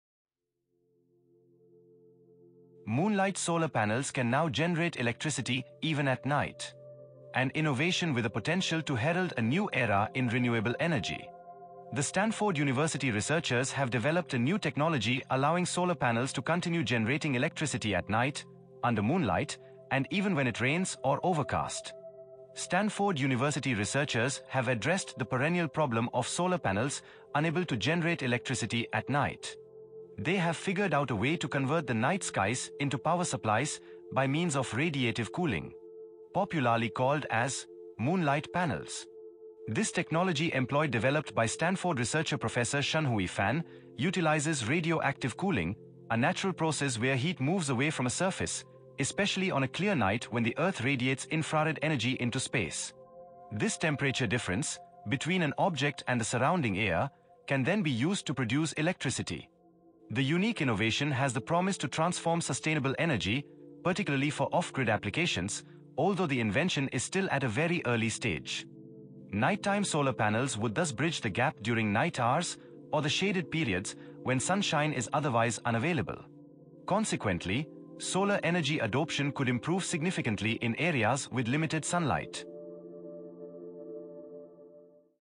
Moonlight Solar Panels: Powering the Night!
Imagine solar panels that don’t stop working after sunset!
Stanford University researchers have developed groundbreaking moonlight solar panels that can generate electricity at night using radiative cooling.
✅ Works under moonlight, cloudy skies, and even rain
✅ Produces energy for LEDs, IoT devices & sensors
✅ Can be retrofitted into existing solar panels
✅ Reduces dependency on costly, polluting batteries
Though the power output is modest today, this innovation has the potential to transform the future of renewable energy and bring reliable electricity to off-grid communities worldwide.
⚡ A step closer to 24x7 clean energy!